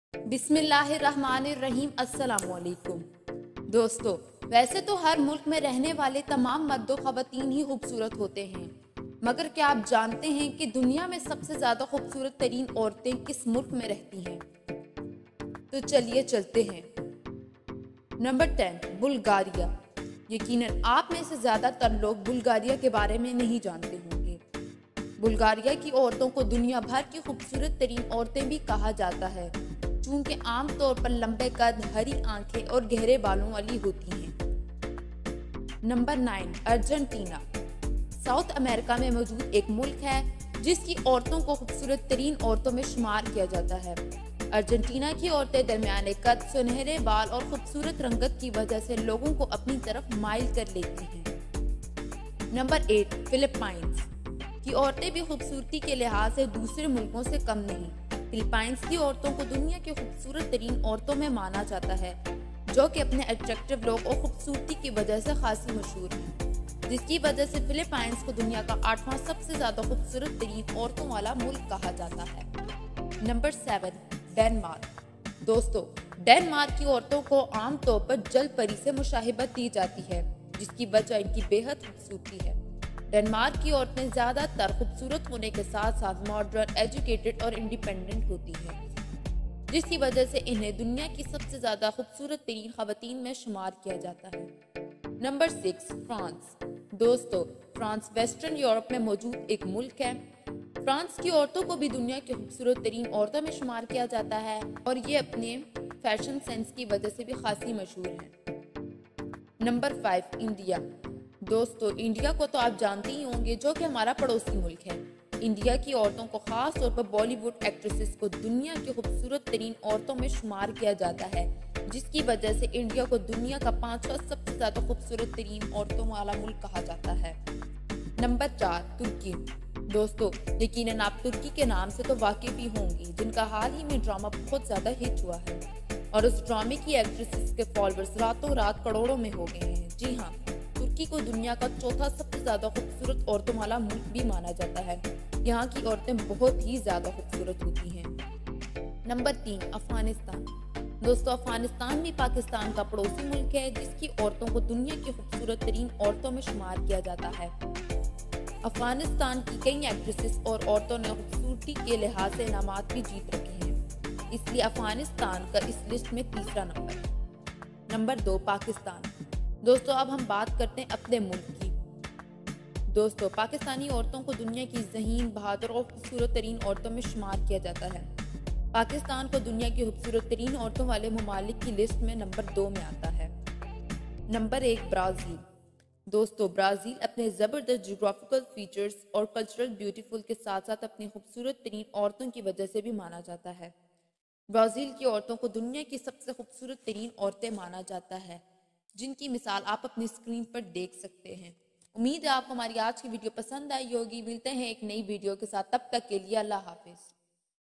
Top 10 countries with most beautiful girls in world || خوبصورت ترین لڑکیوں والے ملک 10 | amir info
Welcome to Amir info tv

Aj mein iss video mein ap ko 10 aise countries ki list bataon ga jahan ki larkiyan dunya mein sub se ziayada khoobsurat hein.